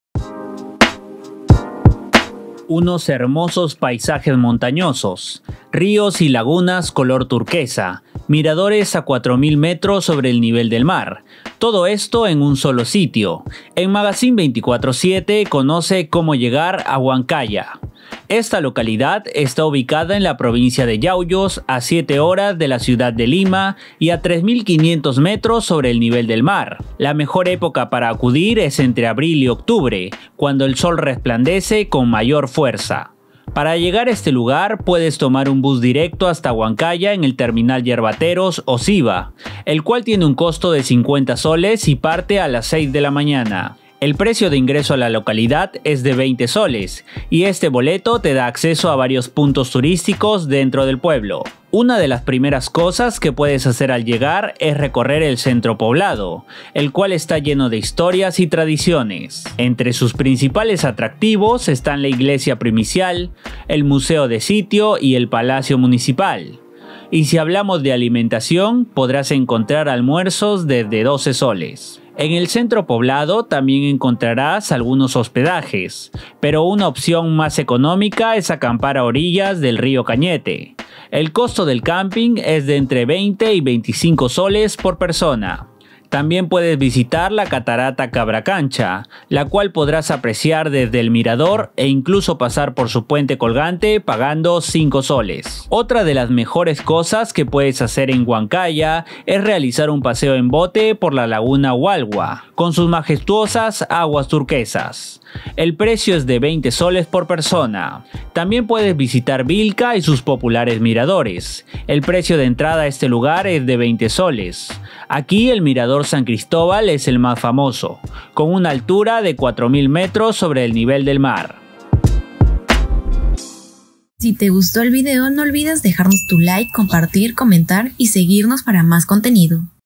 Huancaya: ¿Cómo llegar al famoso paraíso de Yauyos?
Magacín 247 te da los mejores datos para que pases una bella estancia en esta localidad.